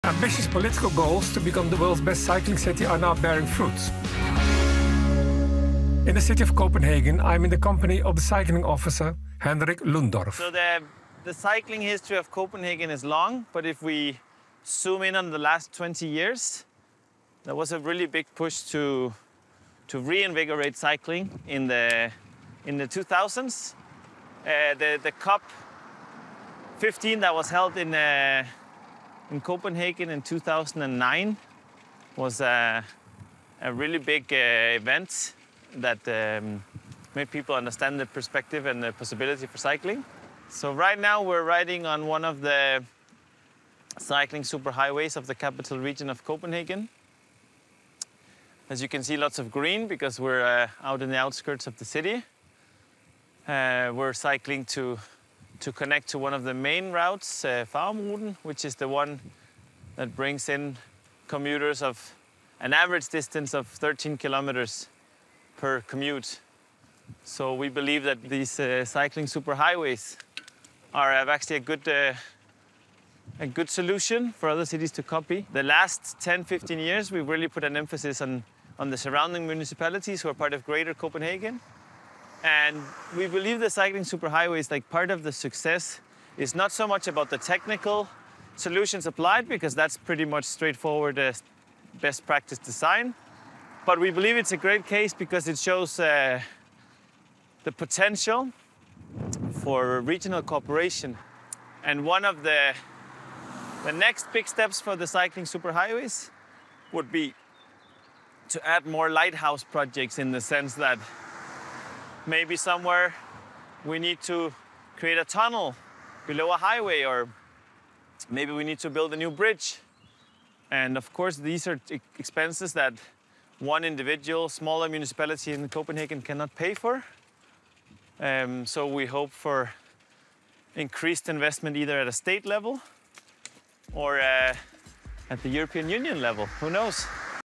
Copenhagen Cycle Super Highways
Henrik Lundorff of the City of Copenhagen  explains how to expand the network of Cycling Super Highways around Copenhagen ad what the European Commission could do.